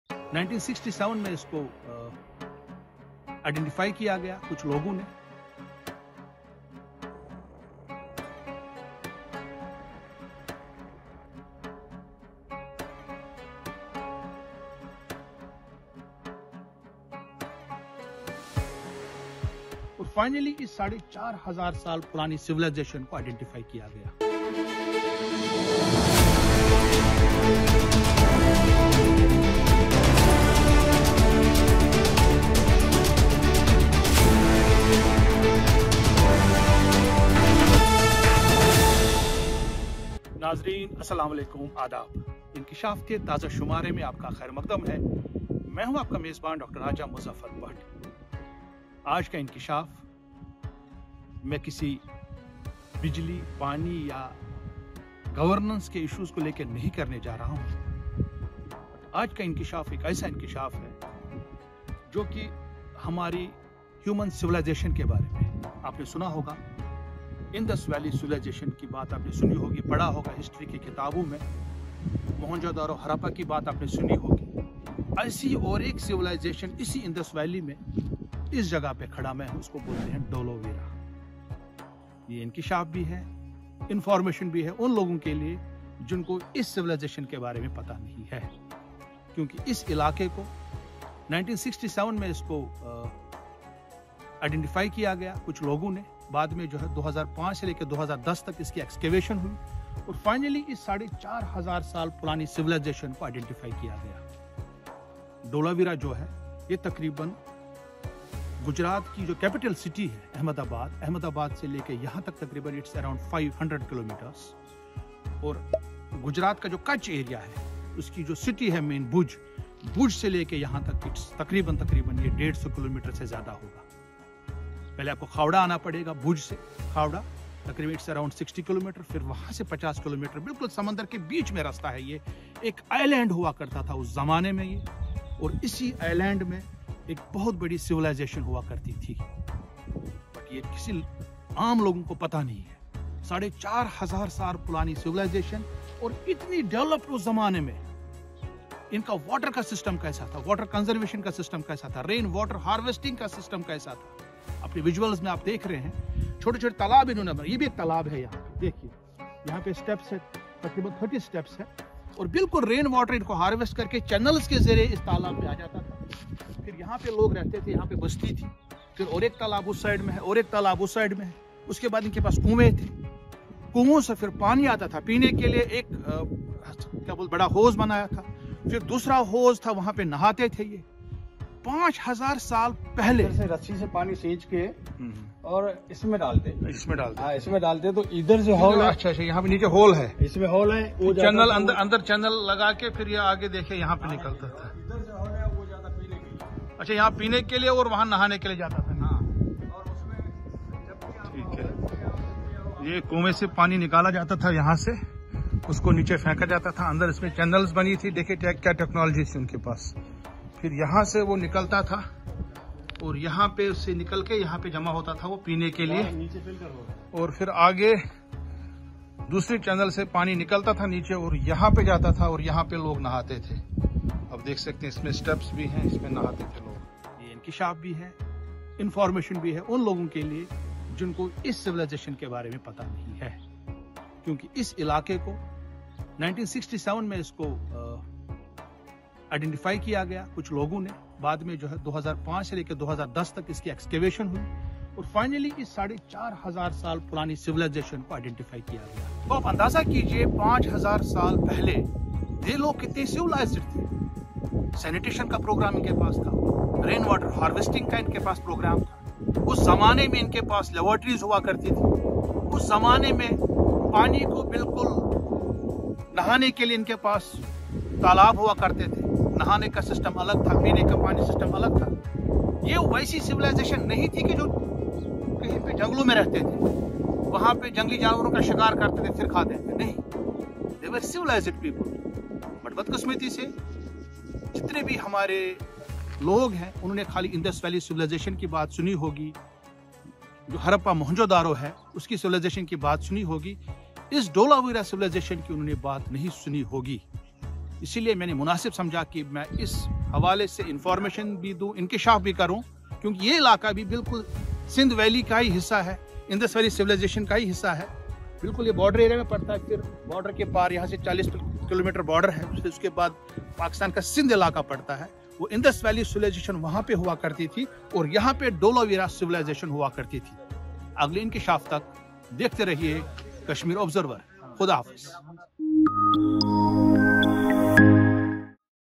Biggest Inkishaf about 4500 years old Civilisation from Dholavira Kutch in Gujarat
Kashmir Observer Team reaches to a remote area near Indo Pak Border in Kutch Gujarat to update you about a 4500 years old civilisation at Dholavira. Watch this week's INKISHAF with Dr Raja Muzaffar Bhat